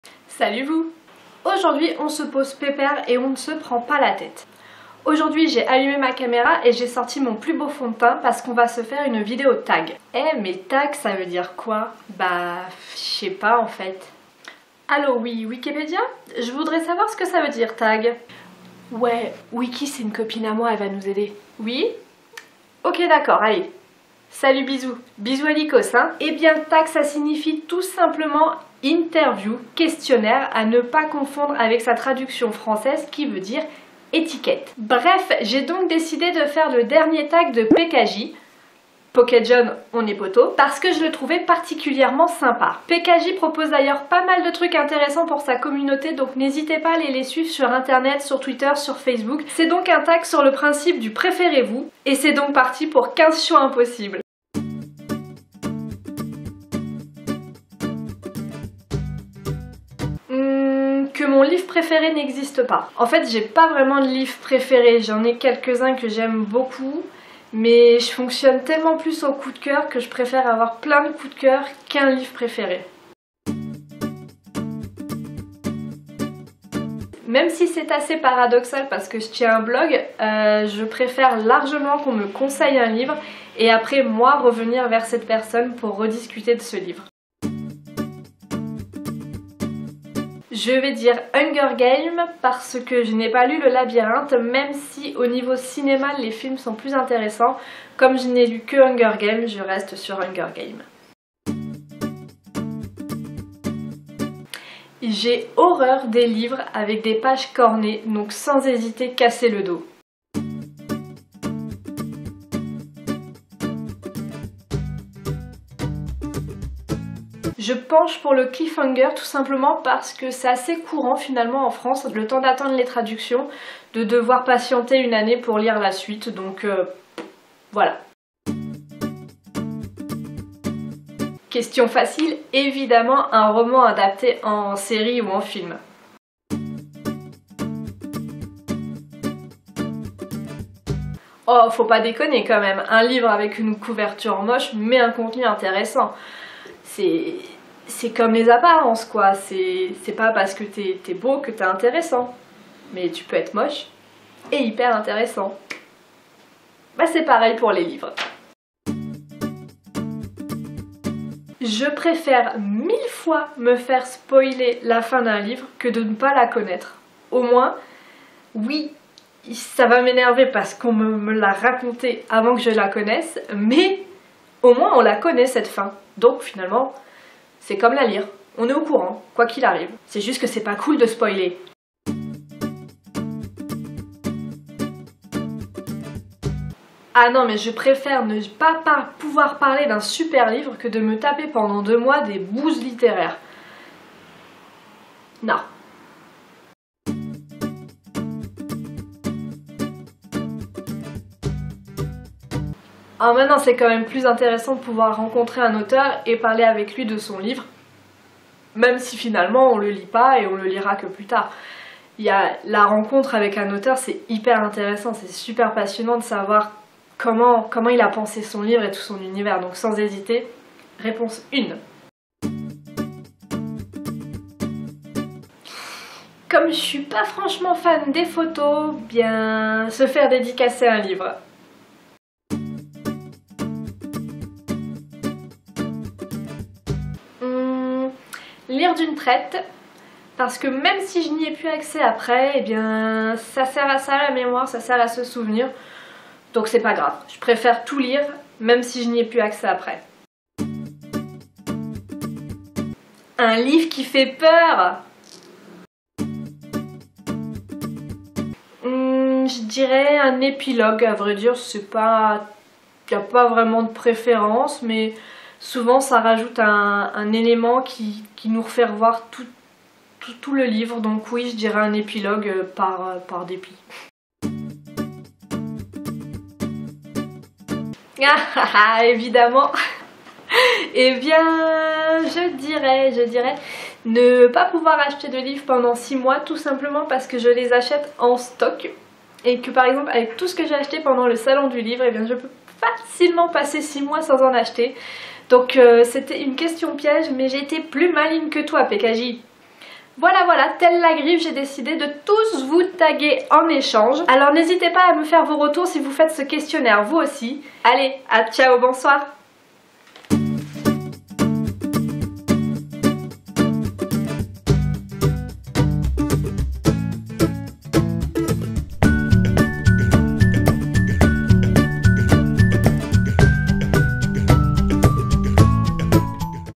TAG PKJ : les choix impossibles #1 | Céline Online
⇩ Clique et tu auras toutes les infos dont tu rêves ! ⇩

Aujourd'hui on se pose pépère et on se prend pas la tête avec le dernier TAG de PKJ (Pocket Jeunesse Super sympa, il s'agit du principe des choix impossibles ("Préférez-vous"). Alors, let's go pour 15 questions bien tordues ^.^

LES QUESTIONS

1) Votre livre préféré n'existe pas OU vous n'avez pas de coup de cœur pendant 3 ans.

2) Conseiller un livre OU se faire conseiller un livre.

3) Hunger Games OU Le labyrinthe.

4) Corner une page de son livre OU casser le dos.

5) Un cliffhanger à la fin d'un premier tome et ne pas pouvoir lire le T2 avant 1 an OU un livre avec une fin ouverte qui n'aura jamais de suite.

6) Un roman adapté en film/série OU un roman non adapté.

7) Un livre génial avec une couverture moche OU un livre passable avec une couverture géniale.

8) Se faire spoiler un livre OU ne jamais avoir la suite.

9) Ne jamais pouvoir parler d'un super livre OU ne lire que des livres nuls pendant 2 mois.

10) Rencontrer un auteur que vous aimez pour qu'il vous parle de son prochaine livre OU pouvoir livre ce prochain livre un an avant tout le monde, sans rencontrer l'auteur.

11) Faire un photo avec un auteur OU se faire dédicacer un livre par cet auteur.

12) Lire une série géniale d'une traite sans plus jamais y avoir accès après OU attendre 2 ans entre chaque tome de cette série.

13) Un livre triste OU un livre qui fait peur.

14) Un livre avec un prologue OU un livre avec un épilogue.

15) Ne pas pouvoir acheter de livres pendant 6 mois OU avoir une panne de lecture de 6 mois.